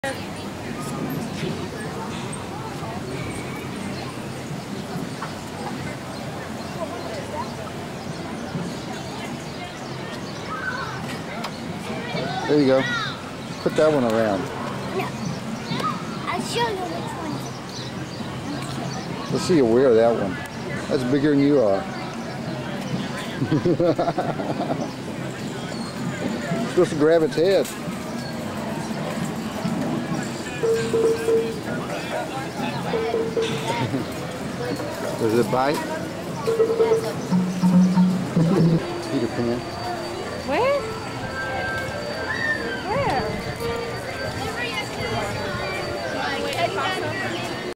Tyler Playing with Snake at Magic Kingdom
Tyler playing with the rubber snakes in Adventureland at Magic Kingdom in 1997.